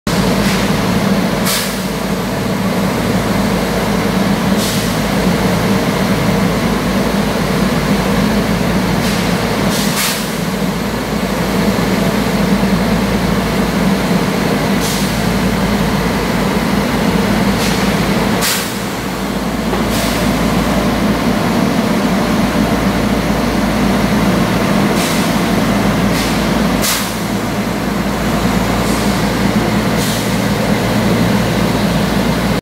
Ms Willem de Vlamingh verlaat de loods - video 1 (8 jan. 2019)
Op dinsdag 8 januari j.l. is de Willem de Vlamingh buiten de loods geplaatst. Dit was best een spannend moment maar het ging erg goed. Credits video: Ingmar Holterman.